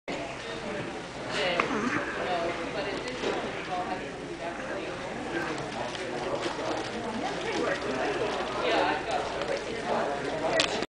Fan Tables at Westercon 74
A portion of the Fan Tables area in the Main Hall of the Tonopah Convention Center at Westercon 74.